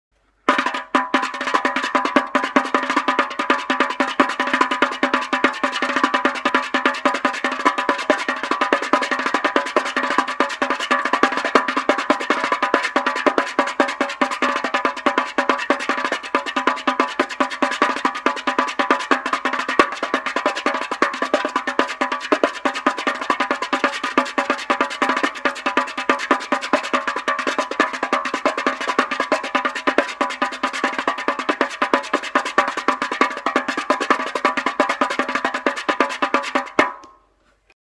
Banjolele drum solo! No strings attached
Whilst setting up a Gibson UB2 I made this little bit of video...
Here's what the Formby style split stroke & fan sound like without strings!  :-)

Visit Andy's website for CDs, downloads and tour dates! Online uke tuition now available - solo lessons or group Zoom workshops... just enquire!